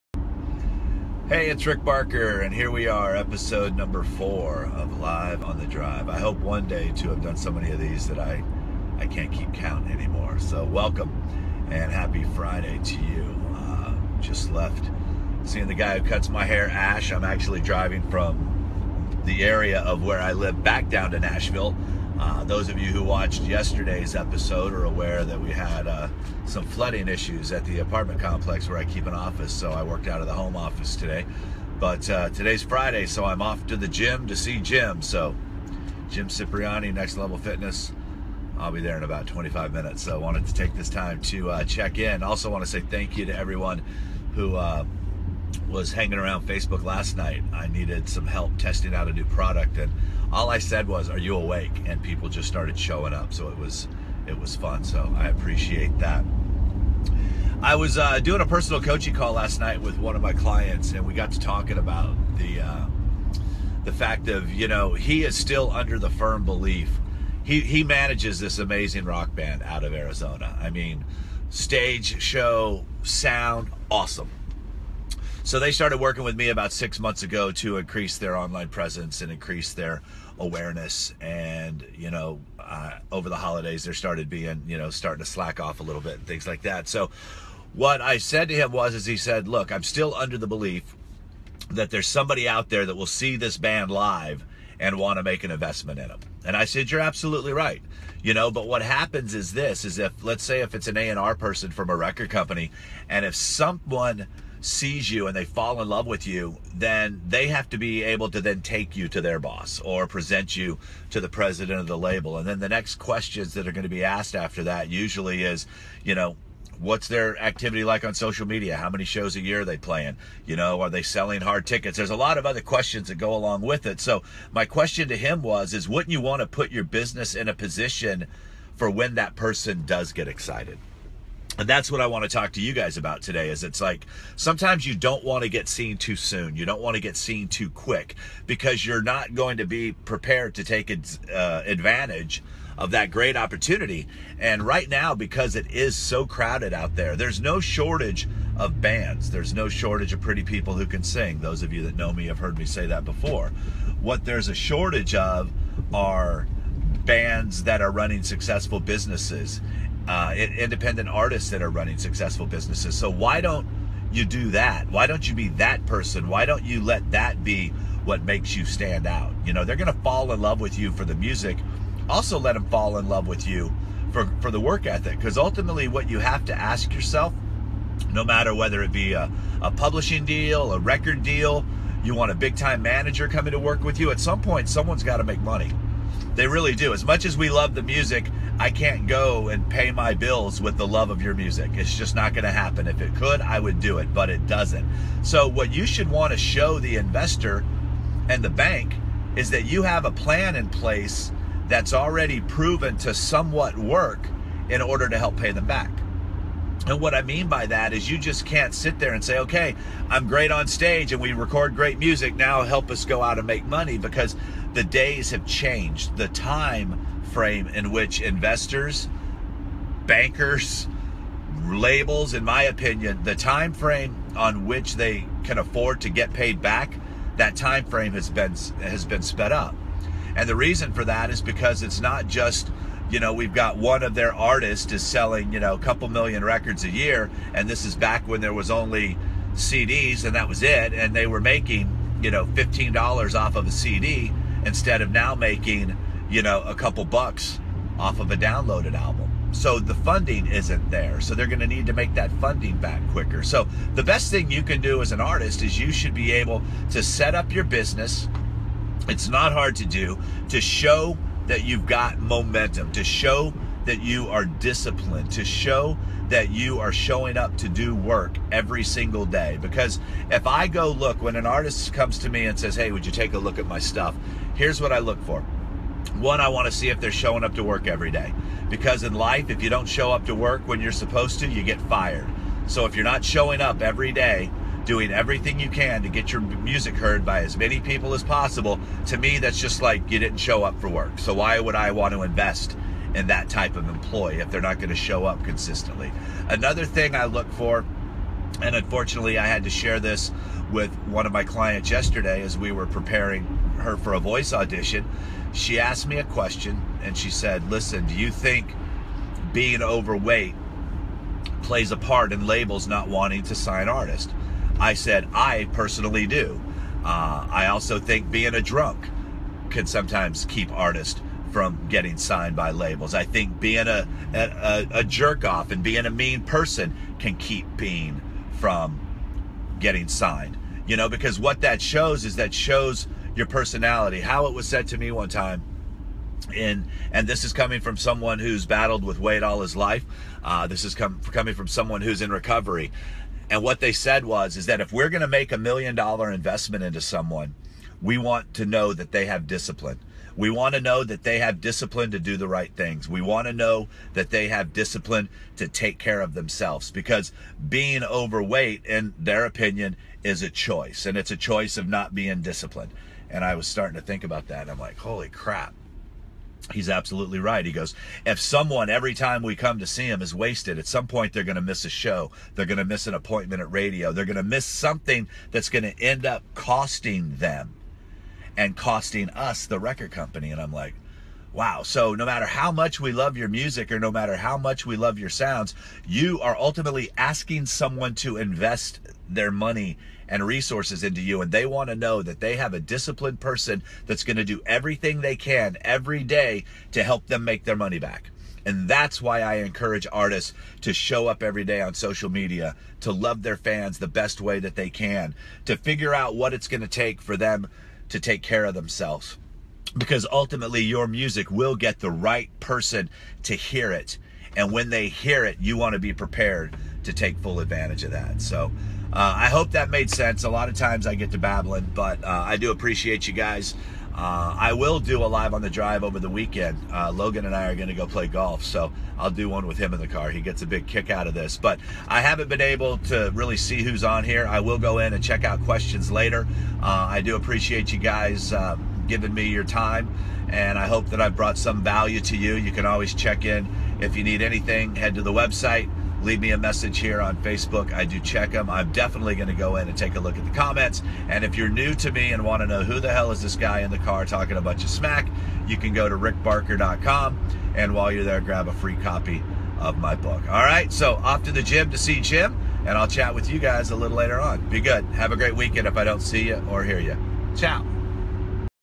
HOW TO STAND OUT | LiveontheDrive with Rick Barker | Episode 4
How can I get an investor to trust me? How can I show that I'm ready and prepared for a record deal?
Be disciplined, be a hard worker, set yourself apart...

Grab a free copy of Rick's book The $150,000 Music Degree